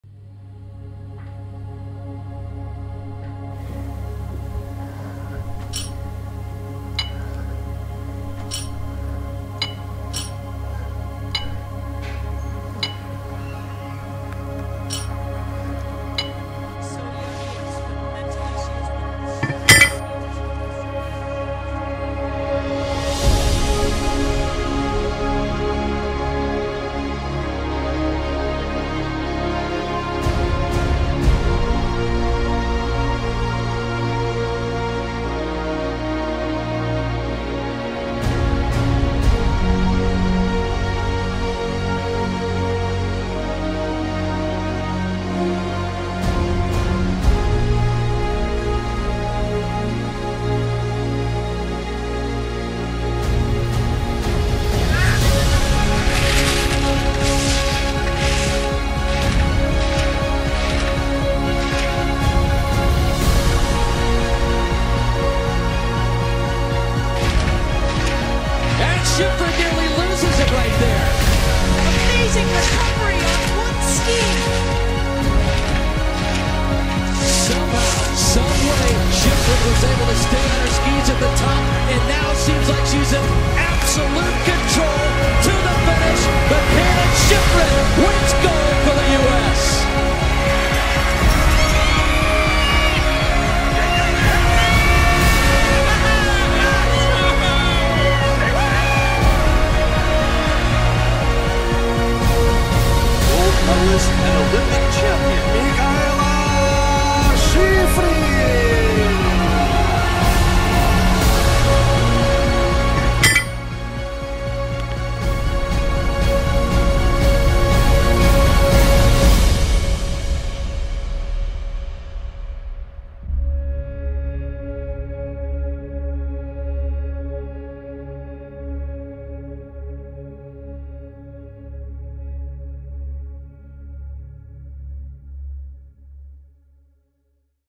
Mikaela Shiffrin • Memories • 2018 [HD]
We wish Mikaela Shiffrin all the best for the upcoming 2018 Winter Olympic Games at Pyeongchang. It's not about the medals, it's about the memories which last forever!

ABFTTB ;-)